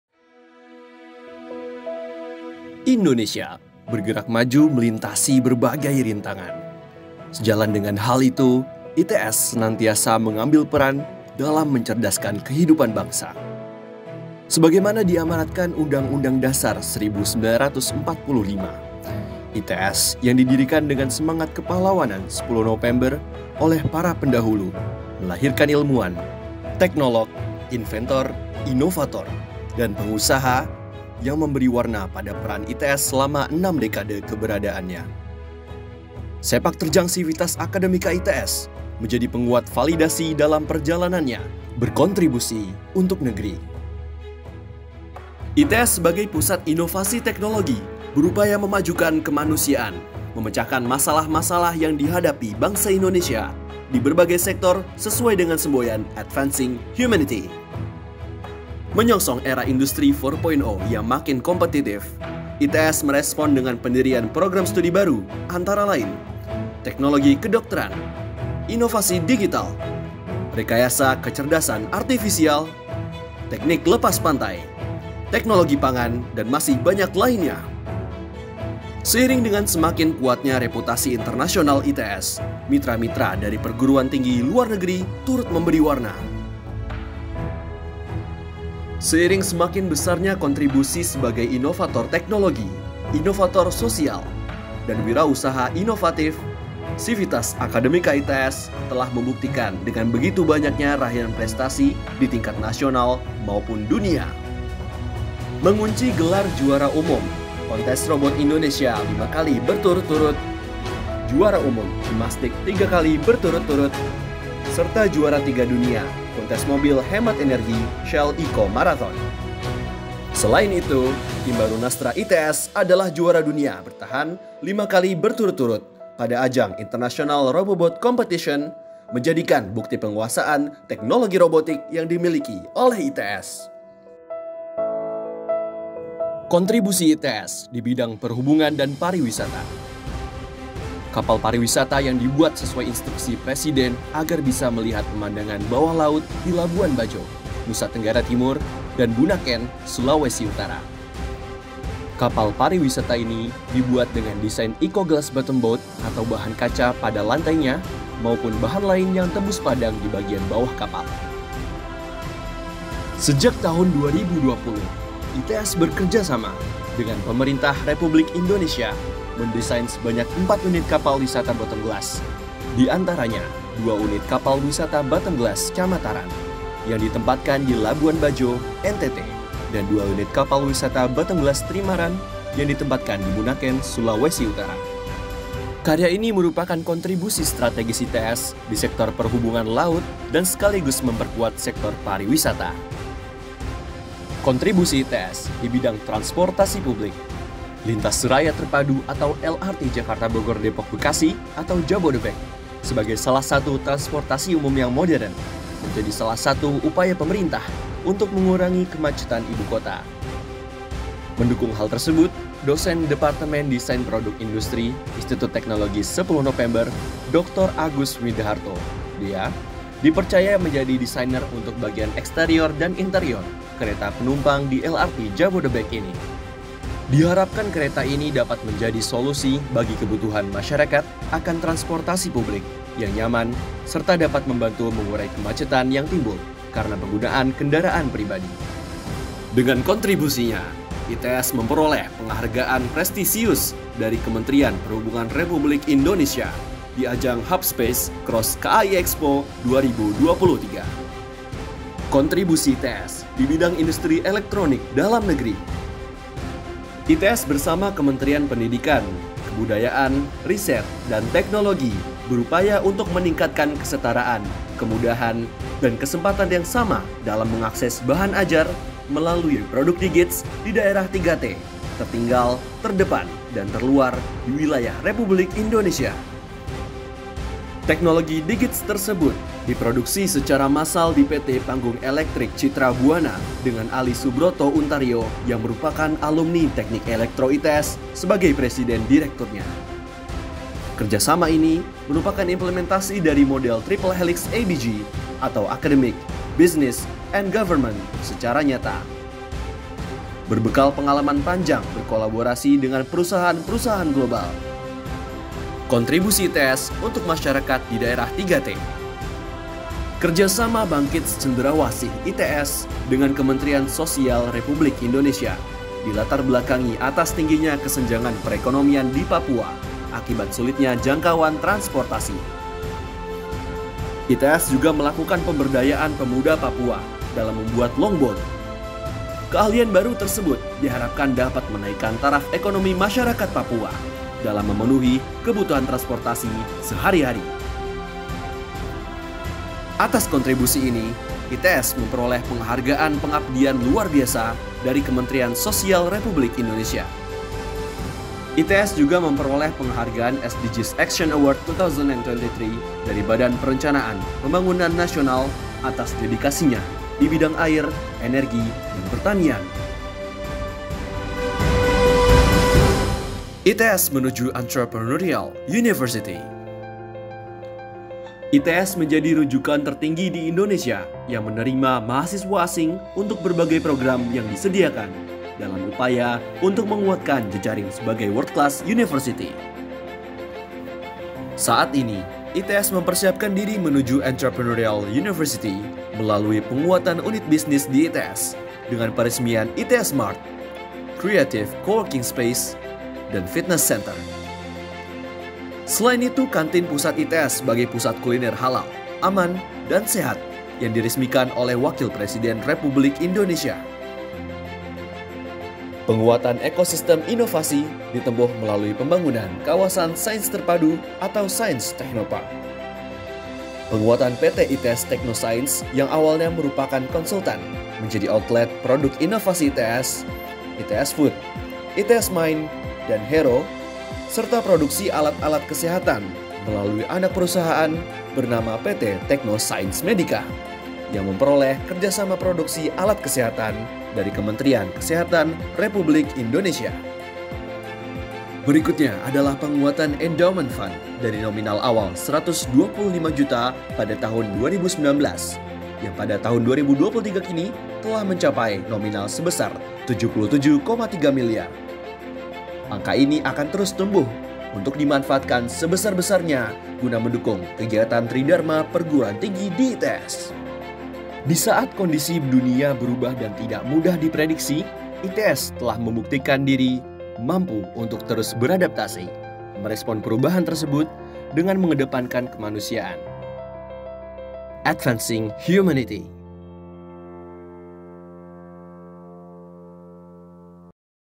Kontribusi 63 Tahun Institut Teknologi Sepuluh Nopember
Sudah 63 tahun berlalu, Institut Teknologi Sepuluh Nopember turut hadir membersamai pergerakan dan kemajuan bangsa Indonesia. Dari Ibu yang luhur ITS, terlahir ilmuwan, teknolog, inventor, inovator, dan pengusaha yang memberi warna pada peran ITS selama enam dekade keberadaannya. Sepak terjang sivitas akademika ITS menjadi penguat validasi dalam perjalanannya berkontribusi untuk negeri.

Setelah enam dekade berjalan, ITS terus mencetak prestasinya dan membawa harum nama bangsa hingga kancah dunia. Sebagai pusat inovasi teknologi, ITS juga berupaya memajukan kemanusiaan dan memecahkan masalah-masalah yang ada, sesuai dengan semboyan Advancing Humanity. Kontribusi 63 tahun Institut Teknologi Sepuluh Nopember untuk Indonesia terangkum dalam video berikut ini. Selamat menyaksikan!